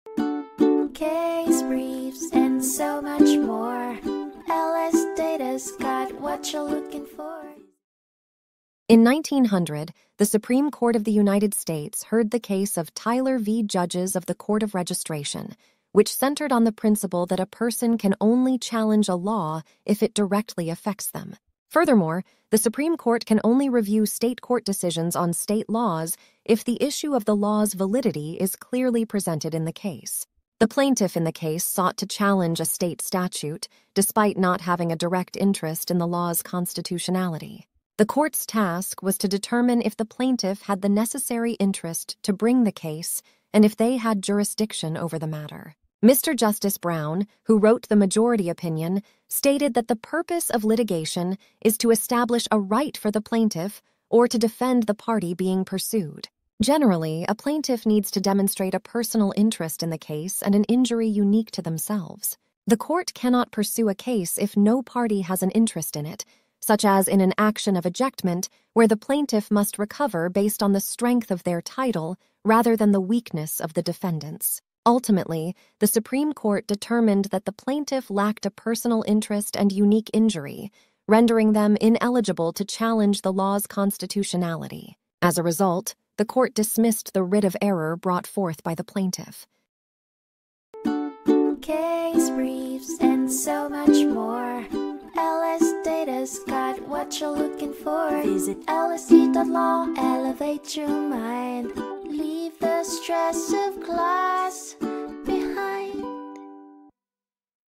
Tyler v. Judges of the Court of Registration (1900) Overview | LSData Case Brief Video Summary
The case is about a legal challenge to a state statute. The plaintiff does not have a direct interest in the statute's constitutionality but wants to challenge it anyway. The court has to decide whether the plaintiff has the necessary interest to bring this case, and whether they have jurisdiction over the matter.

Tyler v. Judges of the Court of Registration (1900)
179 U.S. 405, 45 L. Ed. 252, 21 S. Ct. 206, 1900 U.S. LEXIS 1881, SCDB 1900-043